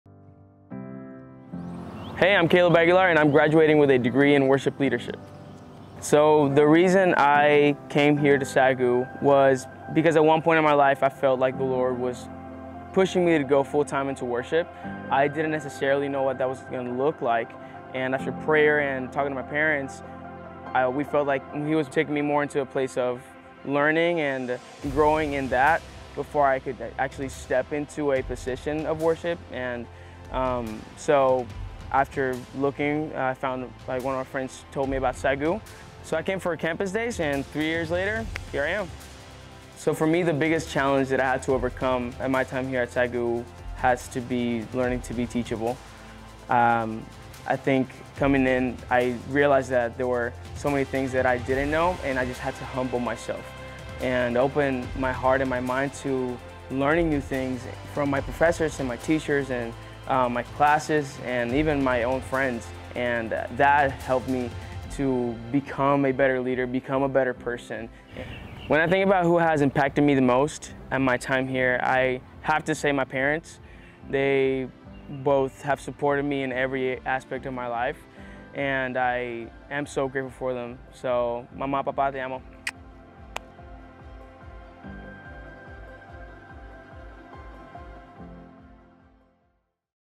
Caleb's Story | A College Student's Journey
Hear from Caleb about his experience at Nelson University.

Want to check it out for yourself?

Sync ID MB01FYEYYXCX0JH